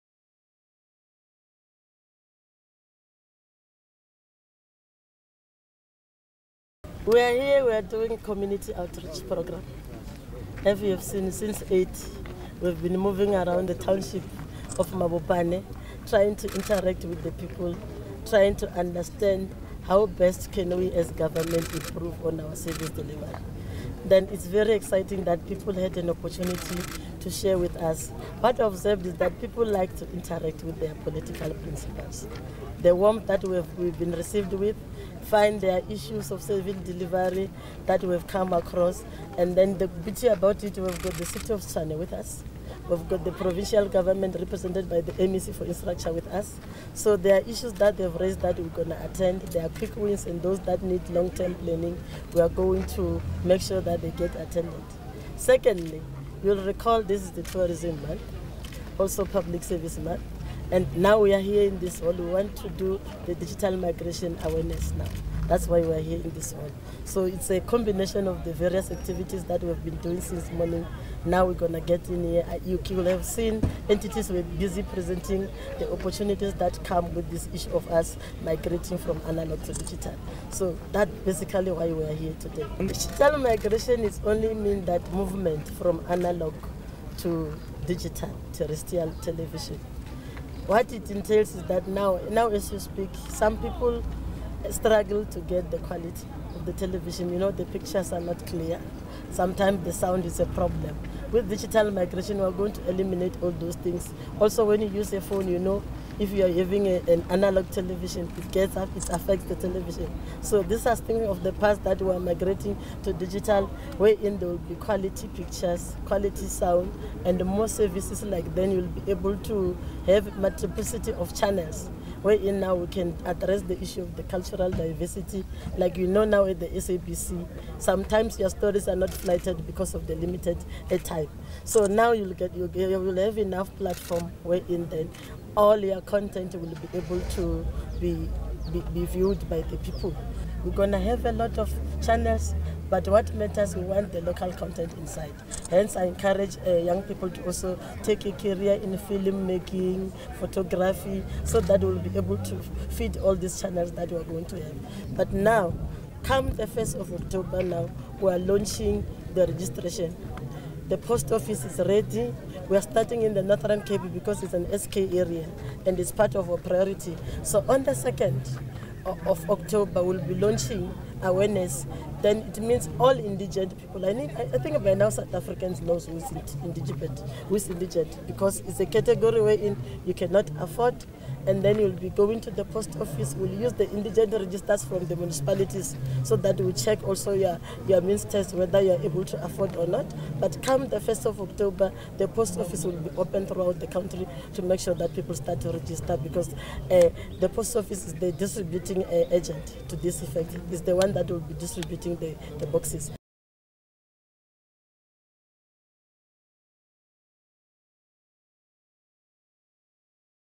Minister Faith Muthambi during Digital Terrestrial Television Community Outreach Programme
Minister Faith Muthambi during Community Outreach Programme on Digital Terrestrial Television (DTT) in Mabopane, Pretoria, 19 September 2015.